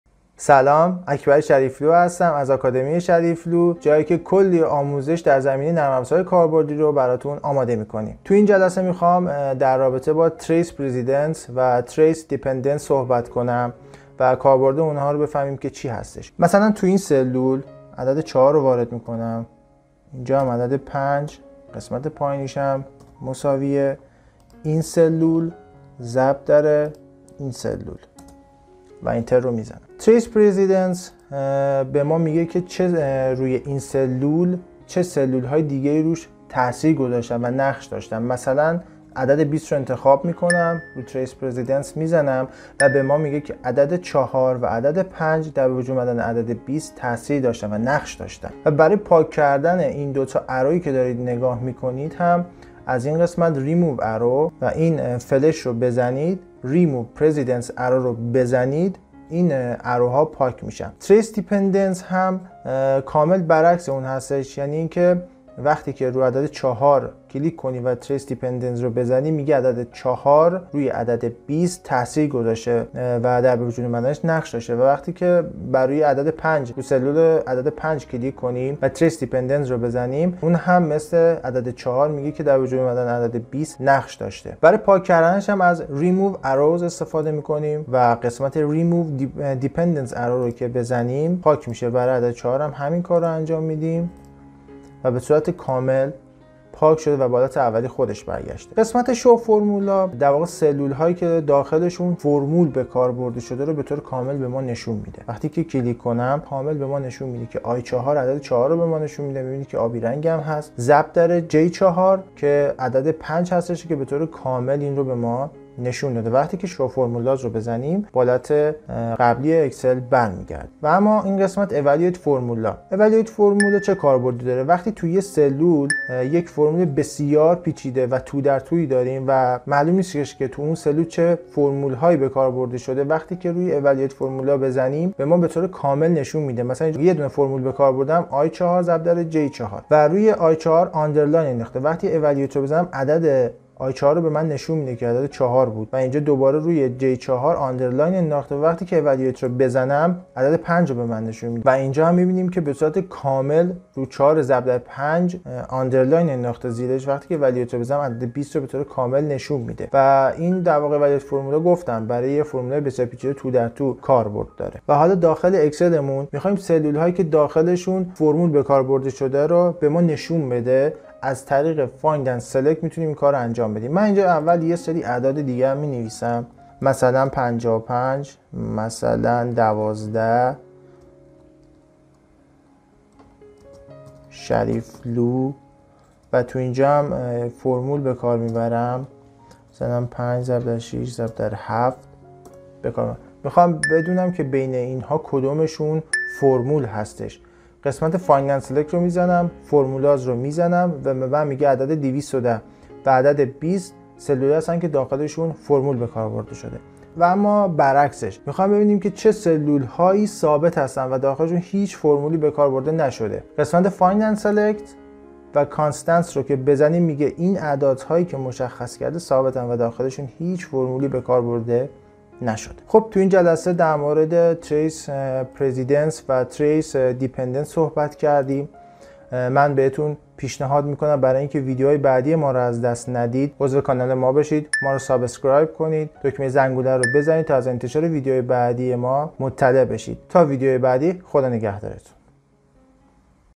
آموزش اکسل - قسمت ۲ - ردیابی آدرس فرمول در اکسل
📌 در این ویدئو از آموزش های اکسل ، می‌خواهیم بدانیم که با کمک Trace Precedents، یک فرمول از چه سلول‌هایی  اطلاعات دریافت کرده است و همچنین با کمک Trace Dependents می‌خواهیم بدانیم که یک سلول در چه فرمولهایی مورد استفاده قرار گرفته است.

📌 برای اینکه ویدیوهای بعدی ما را از دست ندهید، می توانید همین حالا عضو کانال آموزشی "بهرافیک" شوید: 👇